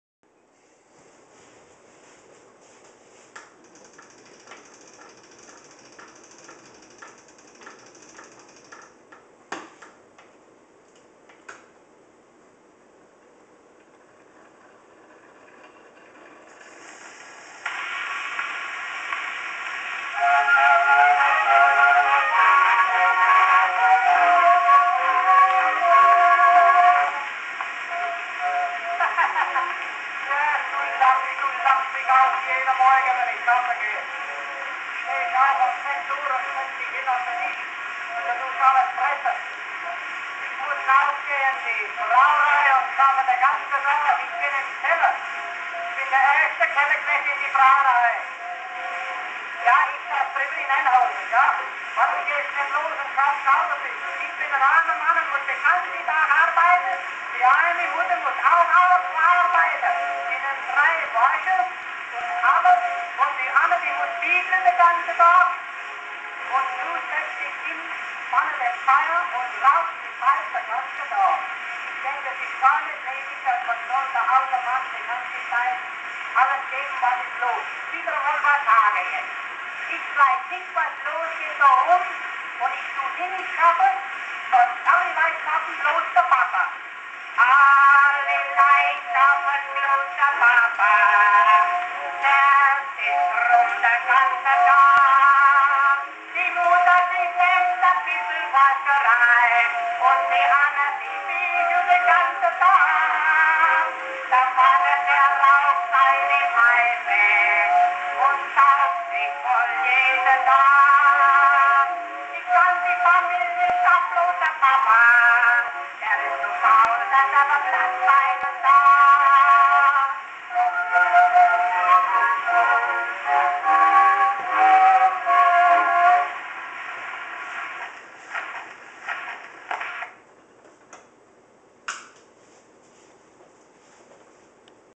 Victor Talking Machine Vic-I Victor the First
Here is an early 8" turntable Victor Talking Machine model: Vic I